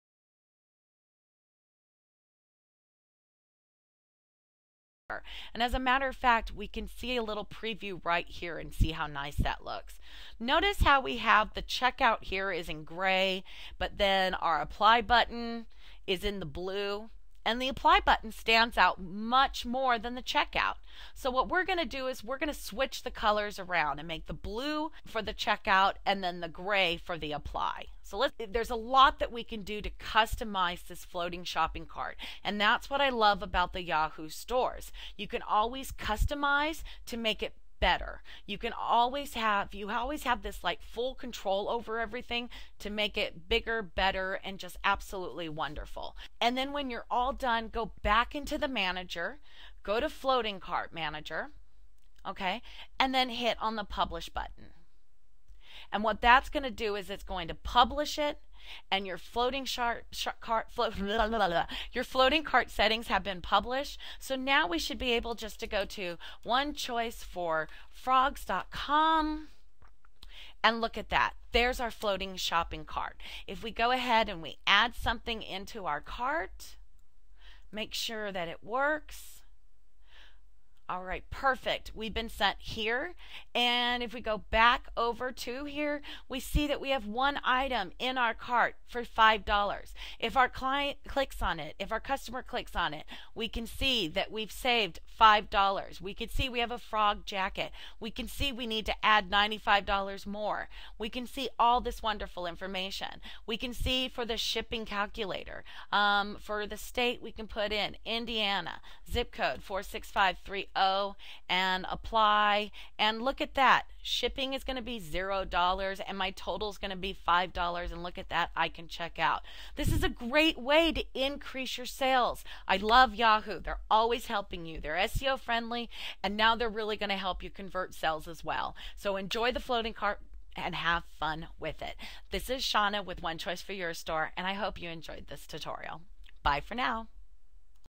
Customizing Your Yahoo Store Floating Shopping Cart Part 2
In our first video, we learned how to install the new Yahoo Store floating shopping cart. However, it is drab, boring, and ugly at first. We can totally customize this cart and make it look absolutely amazing!

Watch this video tutorial walking you step by step through the entire process of customizing you floating shopping cart